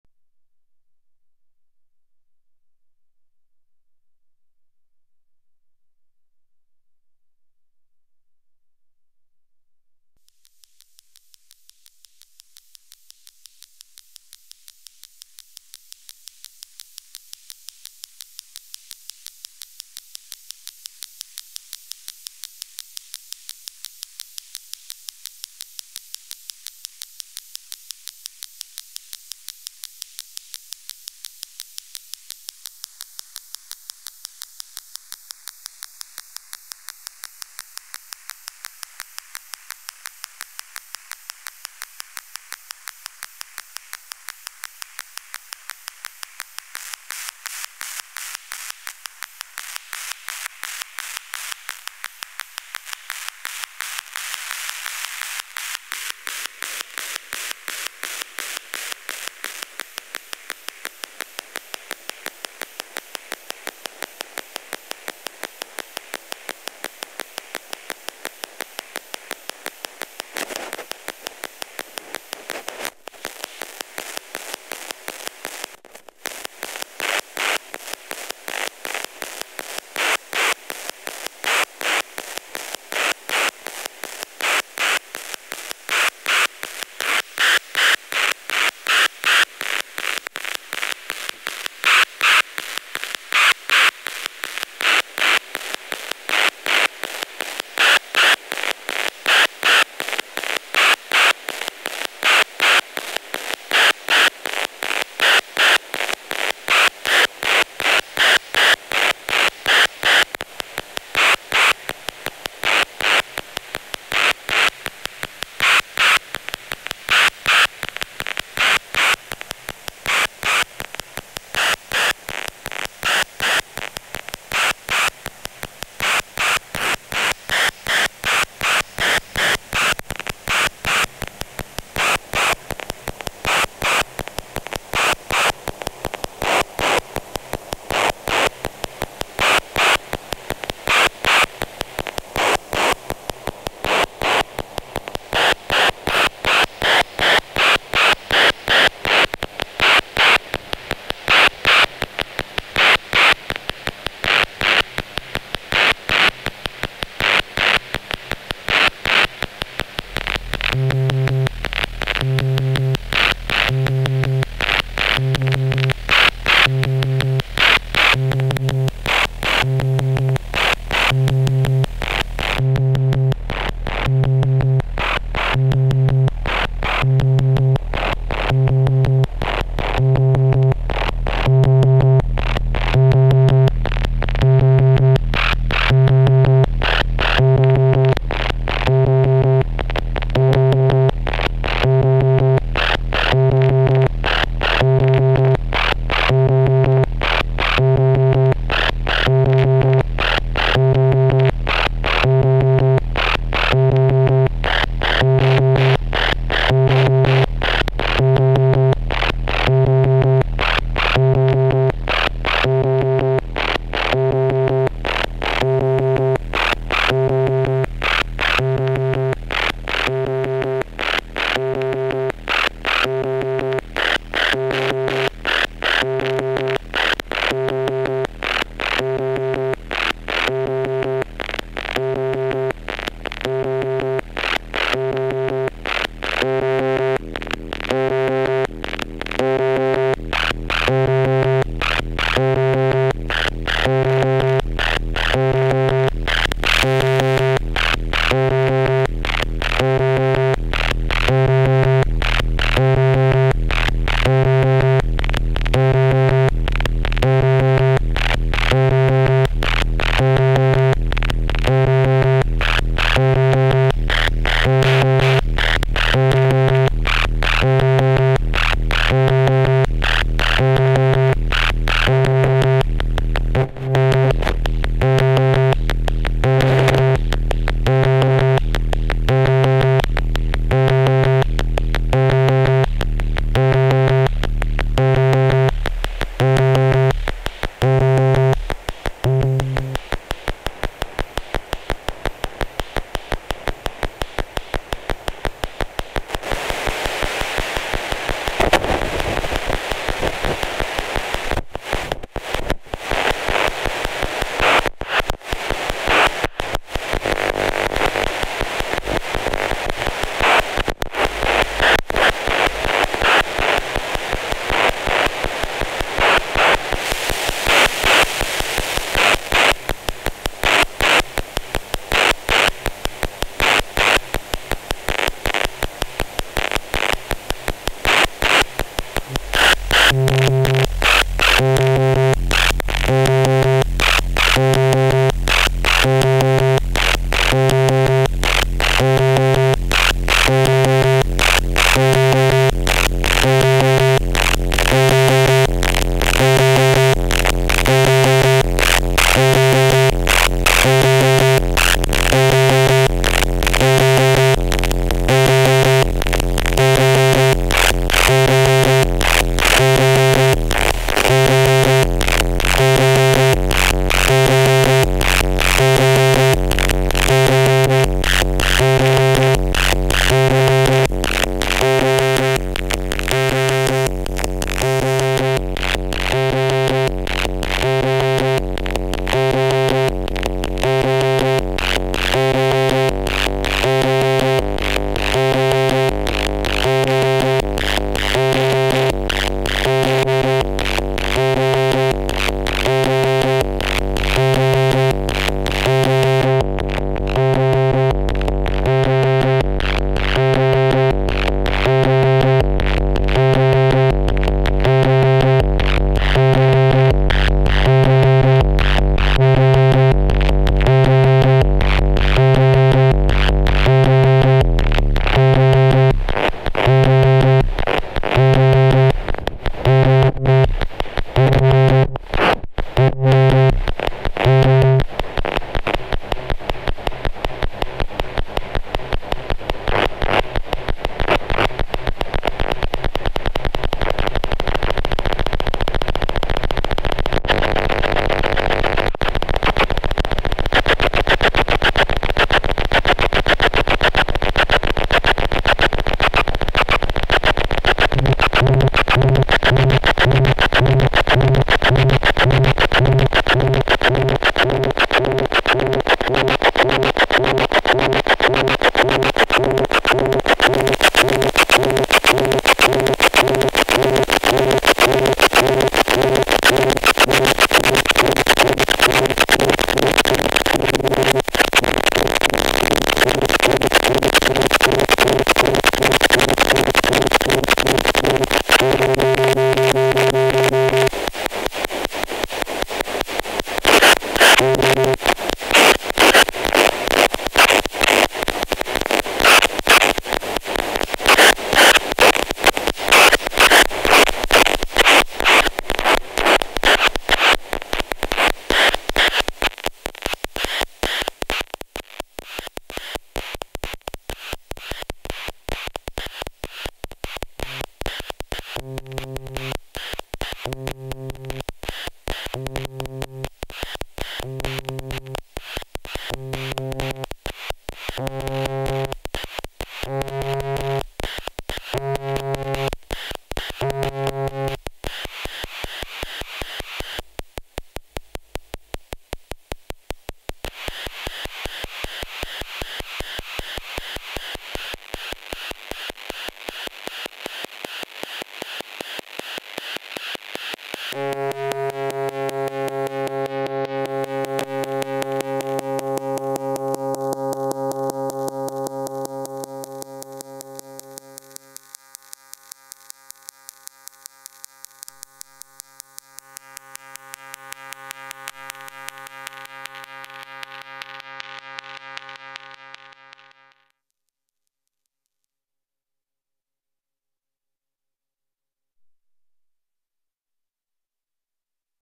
A noisy afternoon with 23 psychoacoustic synth [NO TALK]
Ten minutes with the awesome 23 psychoacoustic + texas multiband overdrive + 8x2cv programmer (8X2cv module is not patched in this video) made by Twintropiques. All sounds come from the synth, no external effects.